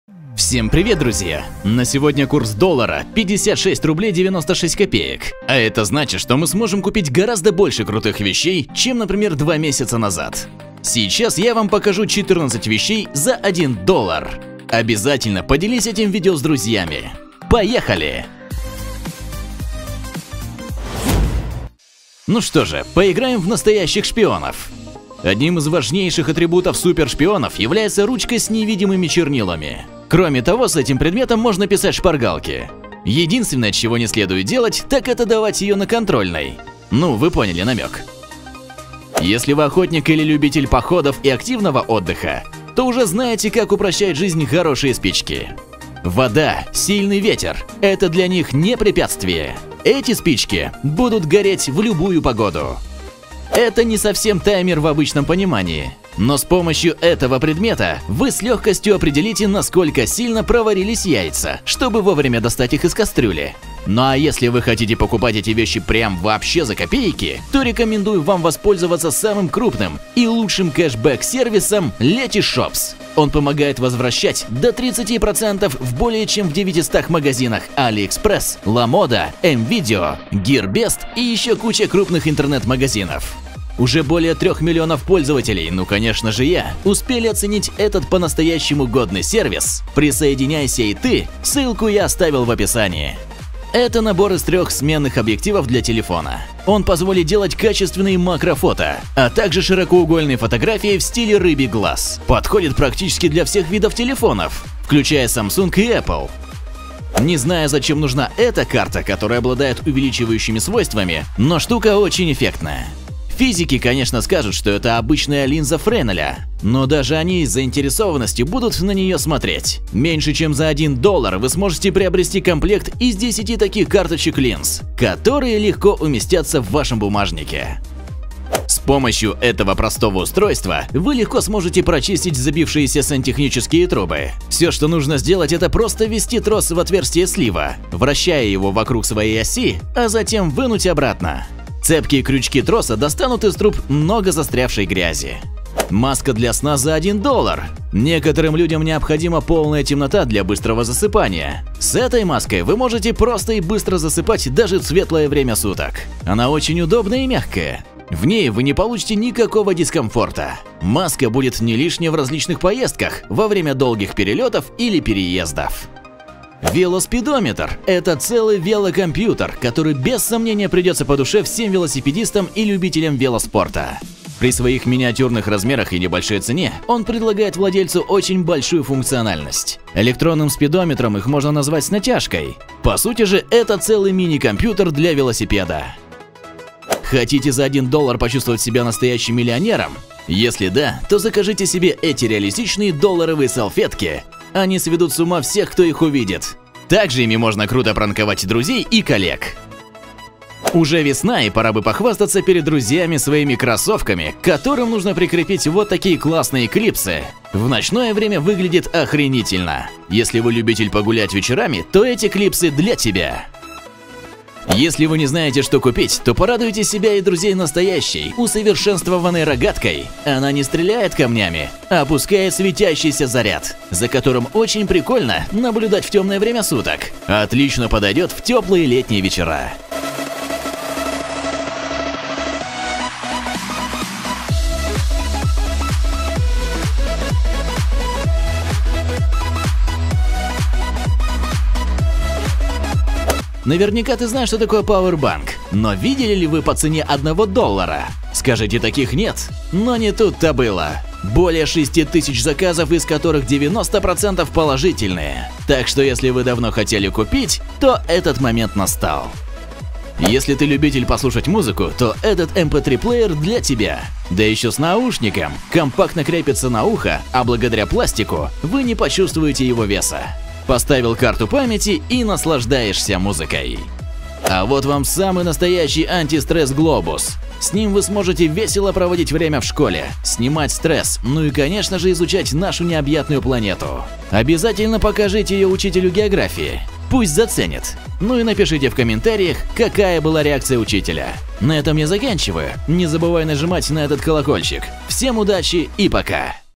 14 НАИКРУТЕЙШИХ ВЕЩЕЙ С ALIEXPRESS до 1 ДОЛЛАРА / ЛУЧШЕЕ С АЛИЭКСПРЕСС
Всем привет, сейчас я вам покажу 14 КРУТЫХ и ДЕШЕВЫХ ВЕЩЕЙ С ALIEXPRESS ДО И ЗА 1 ДОЛЛАР. САМЫЕ КРУТЫЕ ВЕЩИ С ALIEXPRESS. ТОЛЬКО ЛУЧШЕЕ НА АЛИЭКСПРЕСС.